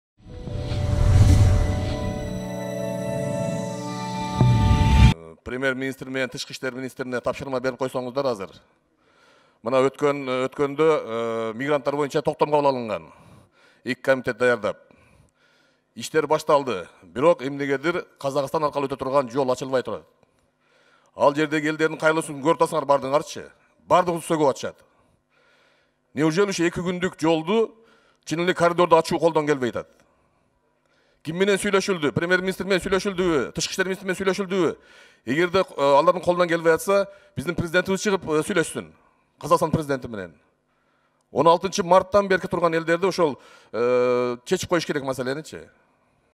Улукбек Ормонов: "Өкмөт орус-казак чек арасында турушкан мекендештерге эмнеге коридор ача албайт?
"Өкмөт россия-казакстан чек арасында 16-марттан бери турган мекендештерге эмнеге Казакстан аркылуу коридор ача албайт? Премьер-министр менен Тышкы иштер министри эмне иш кылып жатышат? Эгер колуңардан келбесе айткыла, анда президент Казакстандын президентине чыгып сүйлөшсүн", - деп билдирди бүгүн, 30-апрелде депутат Улукбек Ормонов Жогорку Кеңештин отурумунда.
Булак: KyrgyzToday.kg